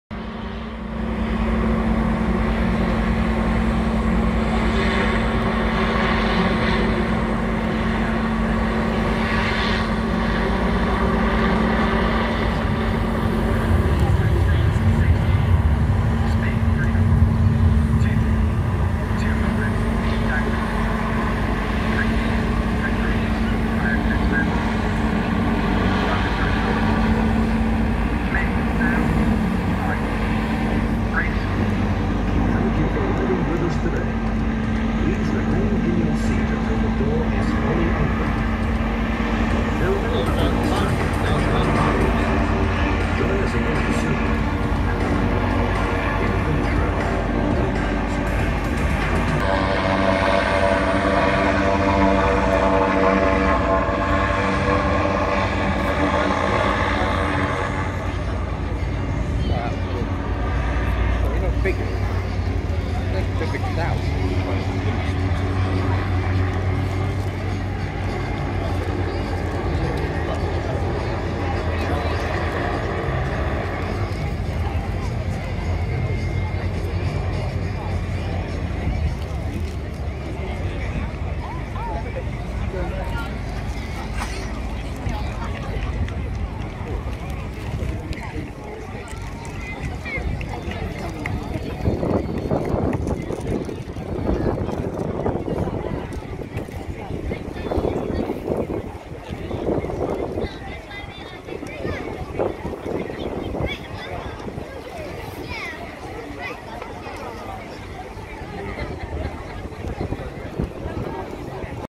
kent Battle of Britain airshow 2021
the Good Year airship dropped in for surprise visit yesterday lunch time